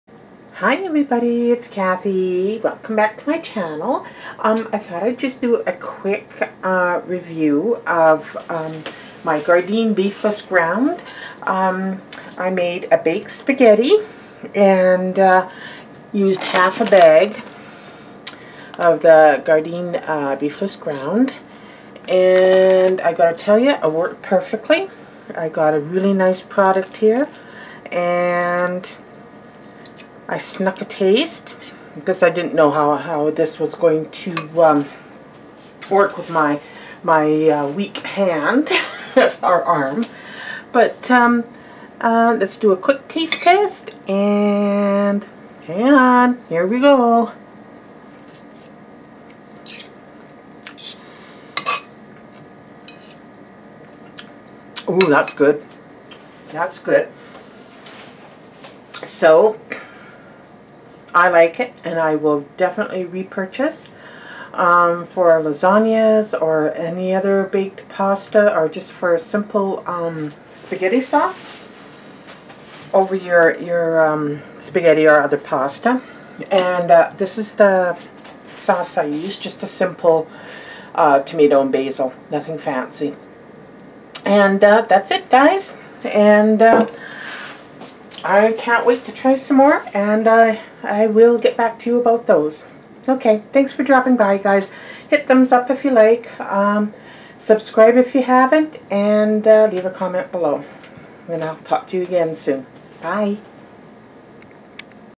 YUM in my TUM
Gardein Beefless Ground taste and review. (baked spagetti)